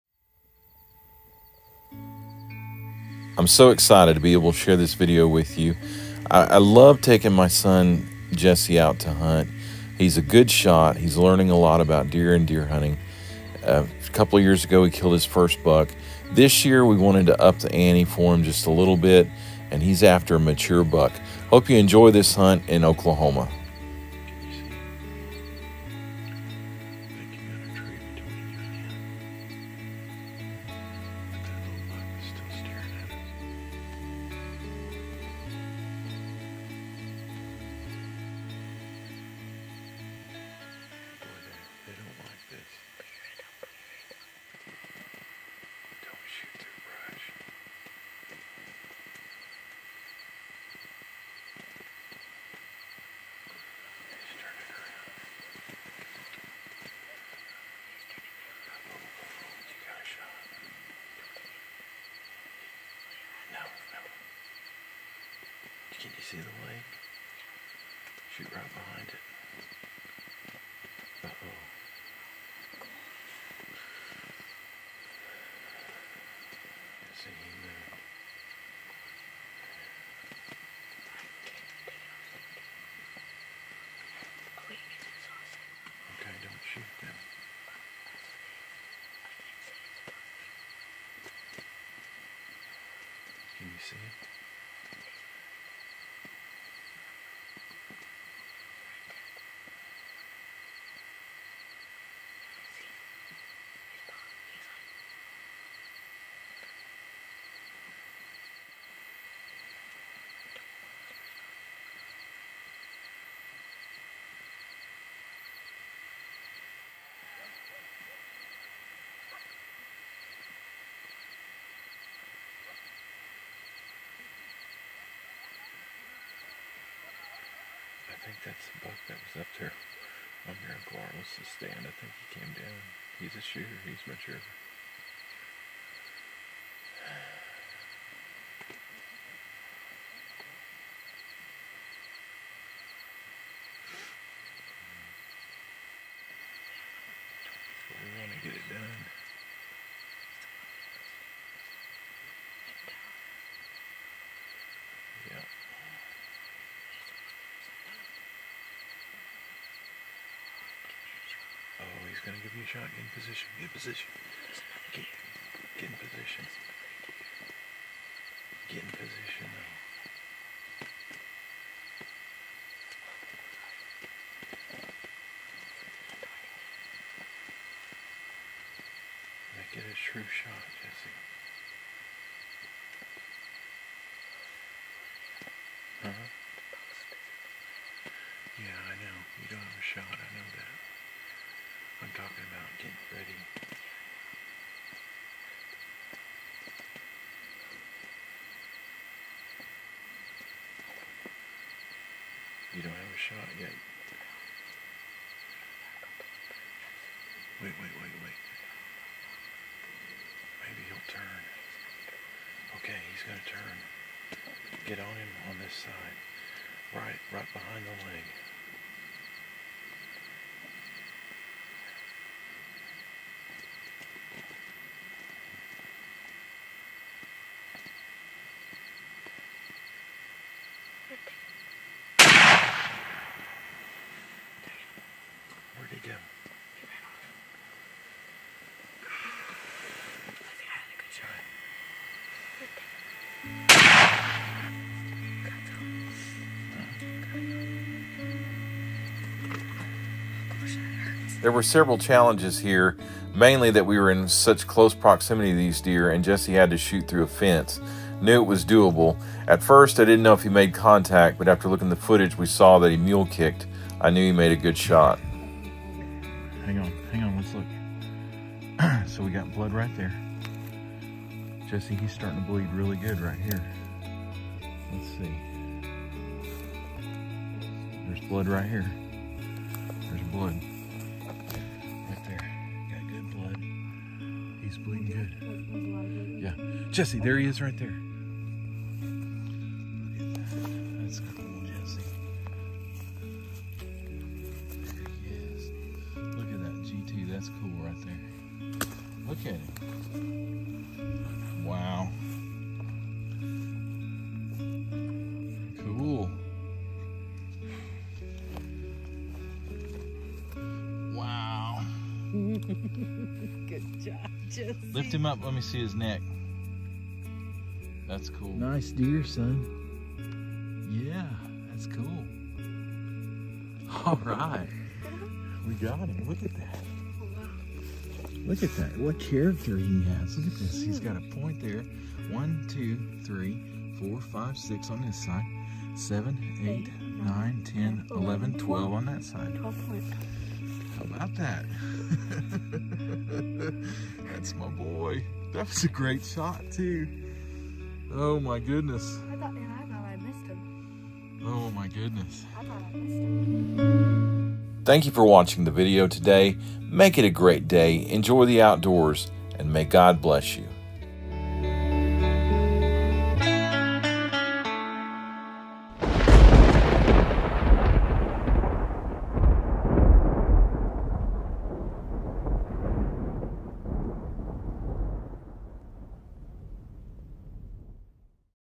12 point Whitetail Buck at 20 Yards.
What a great season my son, Jesse had. I was able to hunt with him as he harvested his best buck.